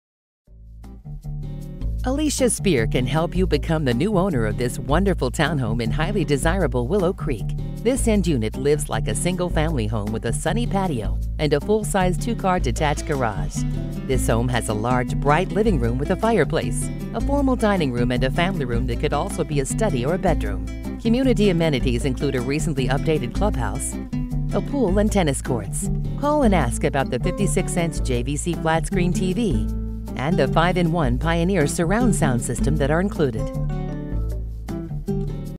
Home for sale in CENTENNIAL, CO | $244,900
WONDERFUL TOWNHOME IN HIGHLY DESIRABLE WILLOW CREEK* END UNIT THAT LIVES LIKE A SINGLE FAMILY HOME WITH SUNNY PATIO AND FULL SIZED 2-CAR DETACHED GARAGE* ROOF LESS THAN ONE YEAR OLD! HOME HAS LARGE, BRIGHT LIVING ROOM WITH FIREPLACE, FORMAL DINING ROOM AND FAMILY ROOM THAT COULD ALSO BE A STUDY/BEDROOM* MAIN FLOOR BEDROOM WITH FULL BATH DOWN THE HALL* UPSTAIRS HAS A LOFT WITH BUILT-IN SHELVING, PERFECT FOR THE HOME OFFICE* THE MASTER BEDROOM IS ALSO UPSTAIRS, PROVIDING PRIVACY FOR THE HOMEOWNER* MASTER BATH WITH OVERSIZED SHOWER, 2 CLOSETS, ONE A WALK-IN, WITH CLOSET ORGANIZERS* **SELLER IS INCLUDING 56' JVC TV, TV STAND AND 5 IN 1 PIONEER SURROUND SOUND SYSTEM!!** ALSO PROVIDING A BUYER'S HOME WARRANTY FOR PEACE OF MIND* COMMUNITY AMENITIES INCLUDE THE RECENTLY UPDATED CLUBHOUSE, POOL AND TENNIS COURTS* HOME IS WITHIN WALKING DISTANCE OF WILLOW CREEK PARK AND DRY CREEK TRAIL, WITH ACCESS TO THE HIGHLINE CANAL* MINUTES FROM PARK MEADOWS MALL, SHOPPING AND IKEA STORE*